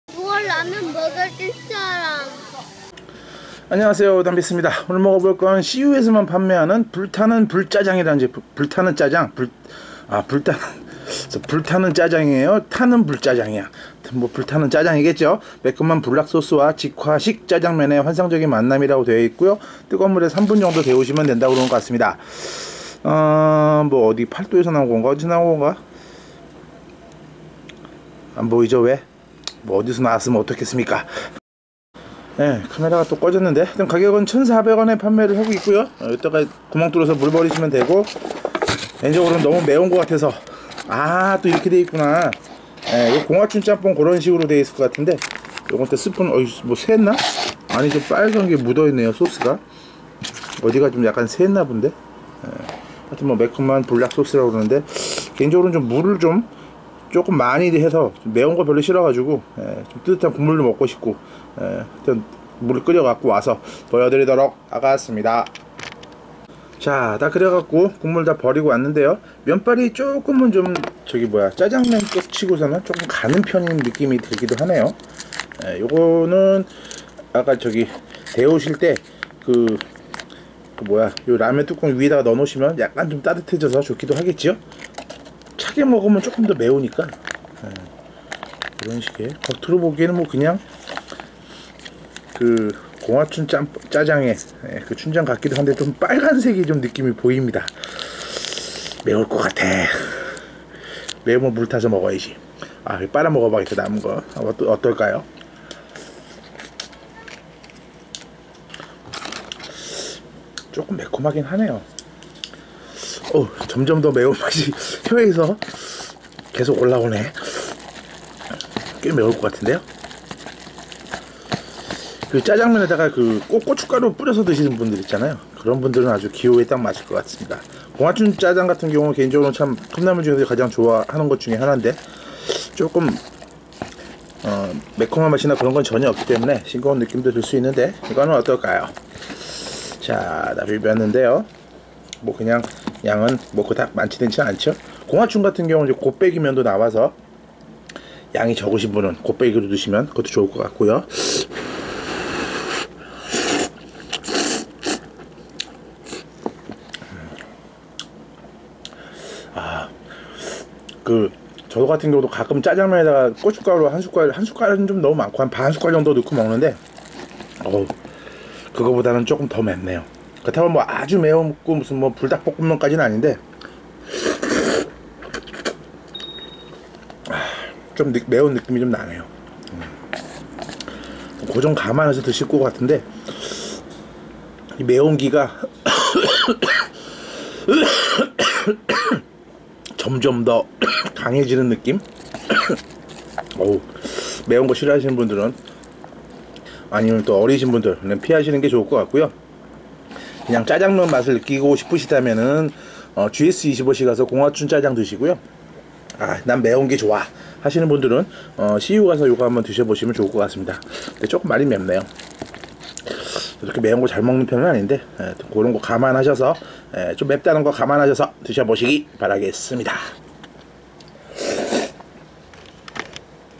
불타는 짜장, CU편의점의 팔도 pb상품 매콤한 짜장라면 시식기
cu 편의점에서만 판매하는 매운 자장면으로 1400원의 가격인데, 아주 맵지는 않지만 좀 매운편으로 짜장면에 고춧가루를 넣어서 드시는 분이라면 좋을듯한데, 매운것을 싫어하시면 gs25 공화춘 짜장면을 추천합니다~